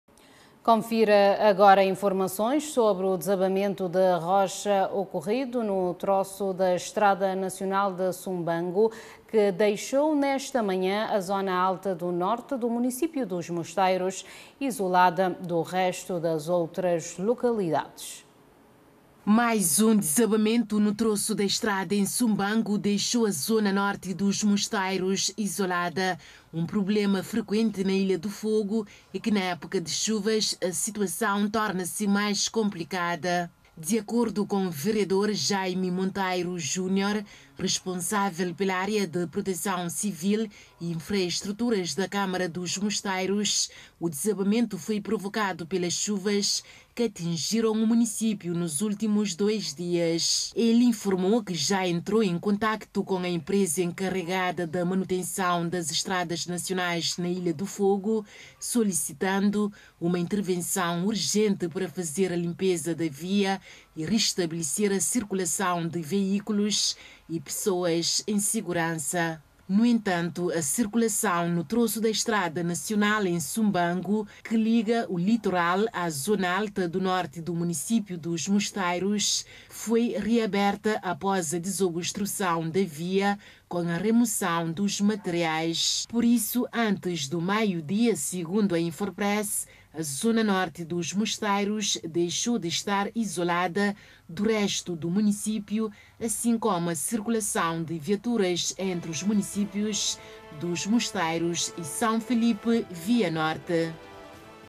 Desabamento de rocha em Sumbango | Fala Cabo Verde
Um desabamento de rocha ocorrido em Sumbango, deixou a zona alta do norte do município dos Mosteiros isolada do resto das outras localidades. Após a desobstrução da via com remoção dos materiais a via foi reaberta.